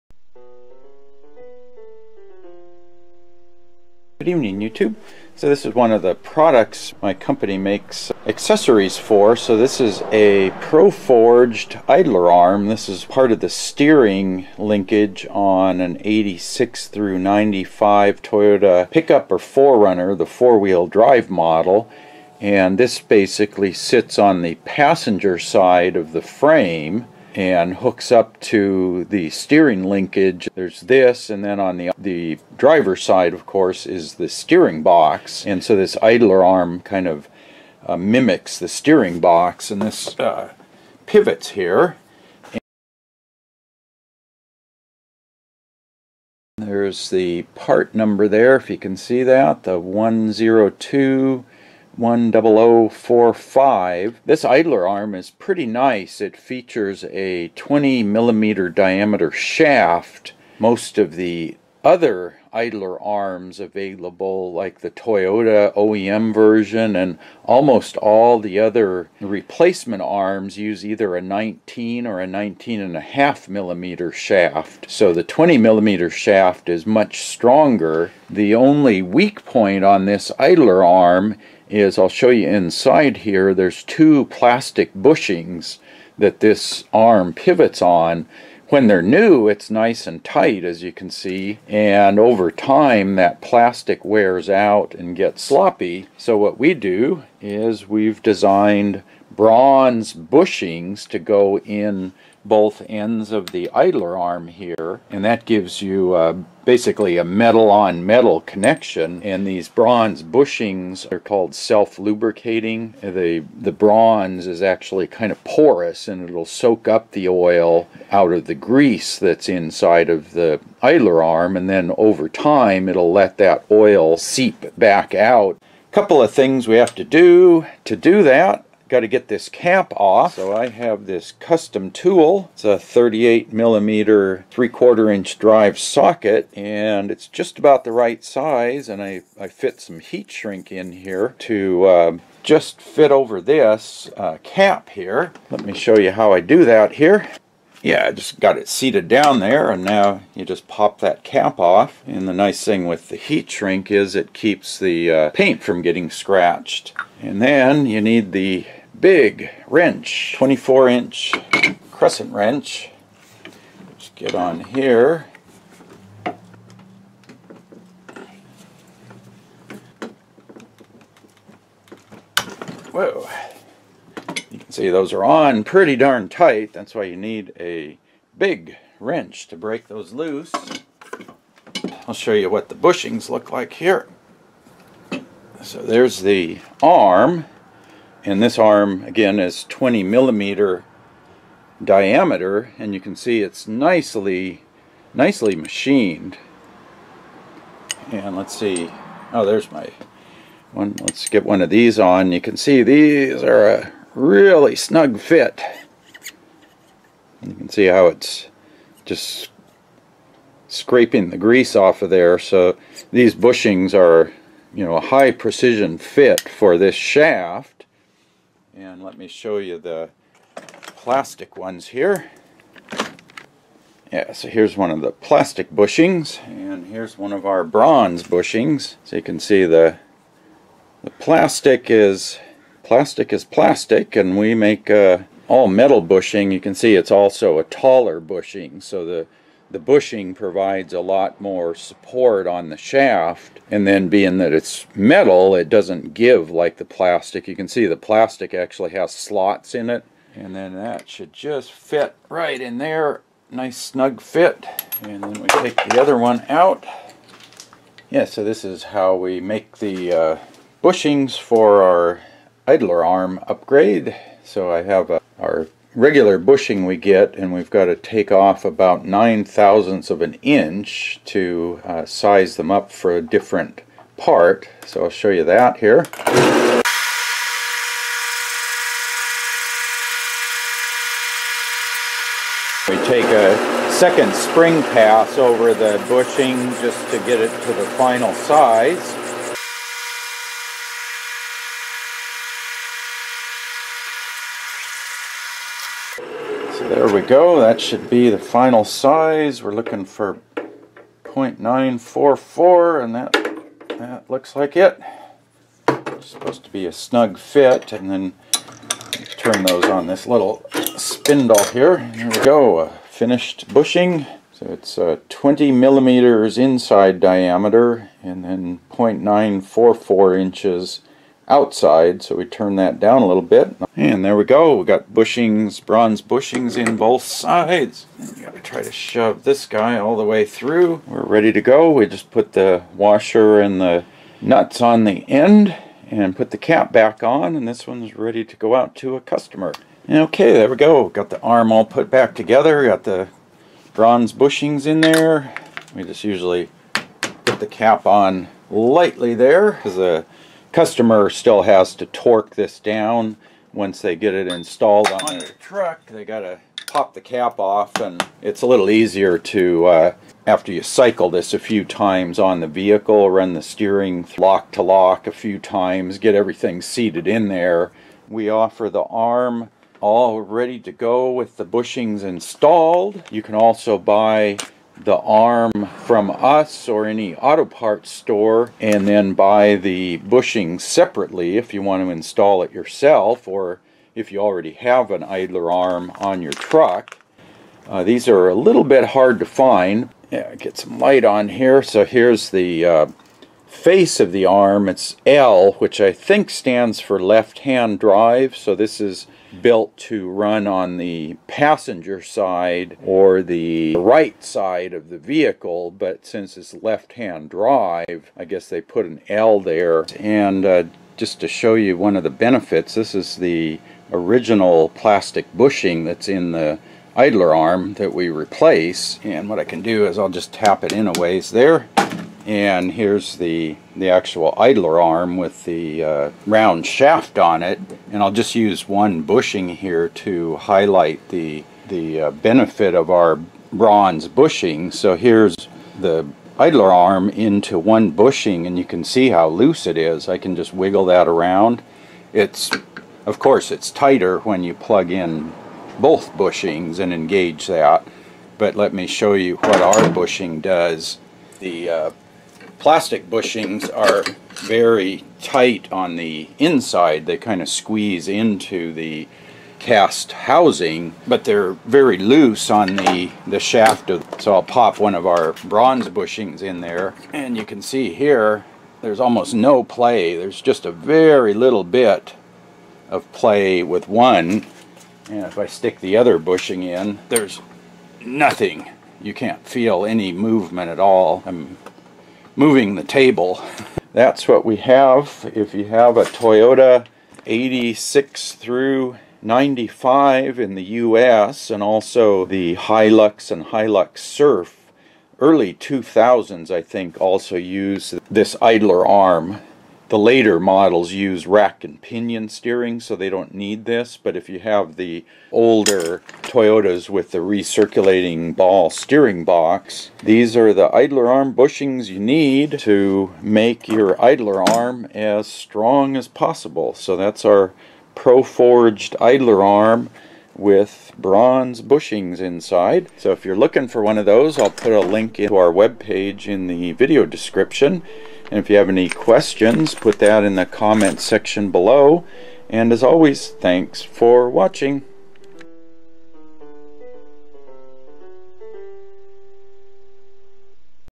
Toyota 4WD/IFS Idler Arm Upgrade, Part 1
Rebuilding a Toyota 4WD/IFS idler arm with bronze bushings.

On the Toyota 4WD pickups/4Runners/Hilux/Hilux Surf models with independent front suspension (IFS), the steering idler arm is one of the most common parts to fail in off road use.  It takes the brunt of the steering force and with the front tires pulling in 4WD mode, those forces can be high enough to bend or break that arm.  Having plastic bushings inside allows for more flexing of the shaft as does having a smaller dia. shaft.  So replacing the idler arm with the largest available shaft dia. as well as high strength aluminum-bronze alloy bushings will give the idler arm the best chance to survive the demands of off road driving.

We originally selected the McQuay-Norris FA5040 idler arm for this upgrade as it featured the larger 20mm dia. shaft and had sufficiently high quality machined finishes to handle the high precision bronze bushings.  Later, this model changed to use a smaller dia. shaft and poor finish.

Then the ProForged idler arm came along that had similar dimensions.  We adapted out bronze bushings to fit the "L" model with it's slightly smaller bushing ID as well as the "zerk" model which uses a slightly thinner flange thickness.

These seem to be the strongest commercially available idler arms and coupled with the bronze bushings, that gives as strong a setup as possible.  There are custom fabricated idler arm replacements that cost 3-4 times the price of this off the shelf setup.